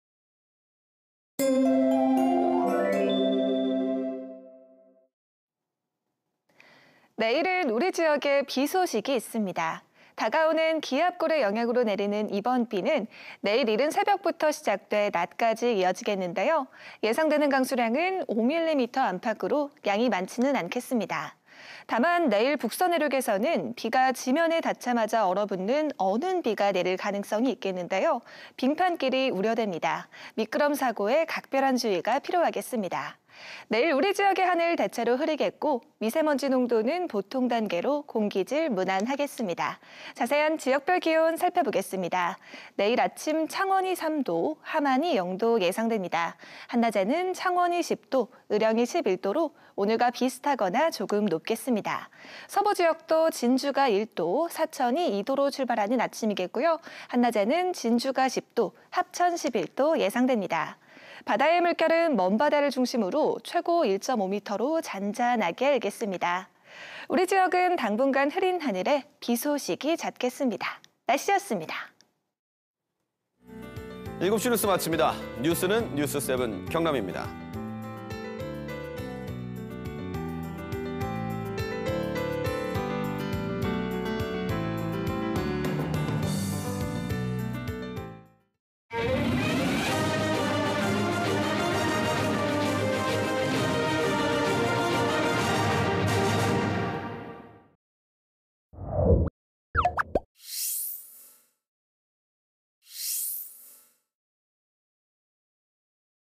[날씨] 경남 내일 새벽부터 가끔 비…예상 강수량 5mm 안팎 / KBS 2024.01.30.
내일은 우리 지역에 비 소식이 있습니다.

다가오는 기압골의 영향으로 내리는 이번 비는 내일 이른 새벽부터 시작돼 낮까지 이어지겠는데요.

예상되는 강수량은 5mm 안팎으로 양이 많지는 않겠습니다.

 다만, 내일 북서내륙에서는 비가 지면에 닿자마자 얼어붙는 '어는 비'가 내릴 가능성이 있겠는데요.

빙판길이 우려되니 미끄럼 사고에 각별한 주의가 필요하겠습니다.

 내일 우리 지역의 하늘 대체로 흐리겠고 미세먼지 농도는 '보통' 단계로 공기 질 무난하겠습니다.

 자세한 지역별 기온 살펴보겠습니다.

내일 아침 창원이 3도, 의령이 1도 예상됩니다.

한낮에는 창원이 10도, 창녕이 11도로 오늘과 비슷하거나 조금 높겠습니다.

 서부지역도 진주가 1도, 거창 -1도로 출발하는 아침이겠고요.

한낮에는 진주 10도, 거창 11도 예상됩니다.

 바다의 물결은 먼바다에서 최고 1.5m로 잔잔하게 일겠습니다.

 우리 지역은 모레 오전부터 오후 사이 다시 한차례 비 소식이 있겠습니다.